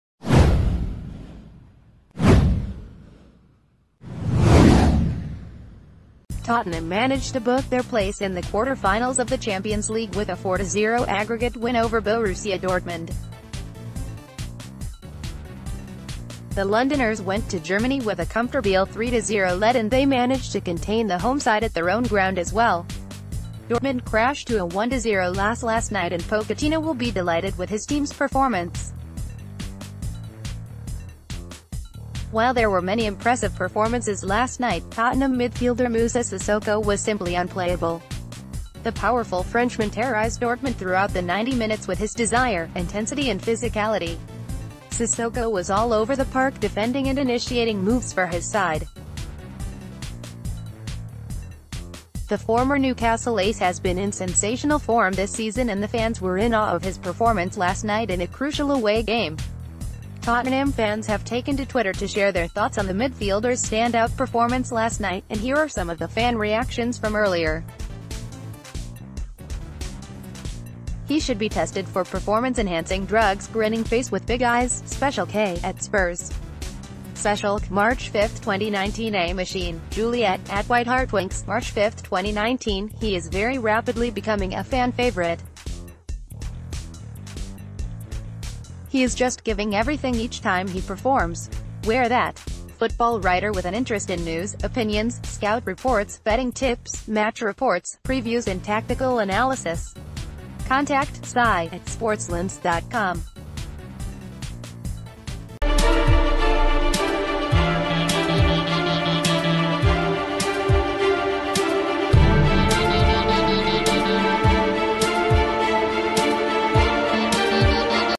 Tottenham fans react to Moussa Sissoko’ display vs Dortmund
Tottenham managed to book their place in the Quarter finals of the Champions League with a 4-0 aggregate win over Borussia Dortmund. The Londoners went to Germany with a comfortable 3-0 lead and they managed to contain the home side at their own ground as well. Dortmund crashed to a 1-0 loss last night and Pochettino will be delighted with his team’s performance. While there were many impressive performances last night, Tottenham midfielder Moussa Sissoko was simply unplayable. The powerful Fren...